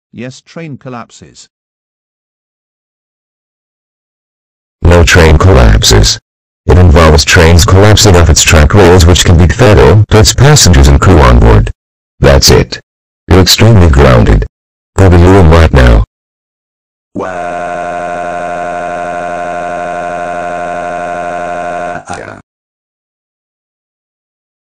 Dark Dreamy Meowser Jr. Says Yes Train Collapses/Grounded
Dark Dreamy Meowser Jr. Says Yes Train Collapses And He Got Grounded By Dark Dreamy Meowser
Next: Dark Giga Dreamy Bowser Jr.